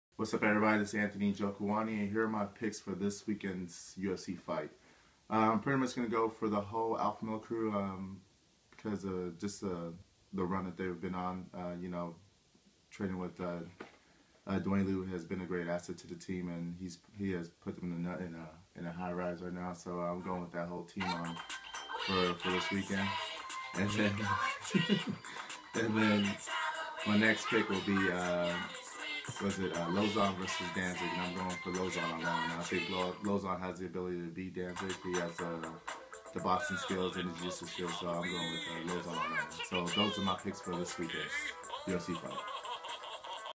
Anthony Njokuani UFC on Fox9 Predictions
UFC fighter Anthony Njokuani makes his fight predictions for the event on Fox Saturday December 14th.  Video powered by Bony Acai and some special music provided as well!! Guest appearance by Kai Njokuani.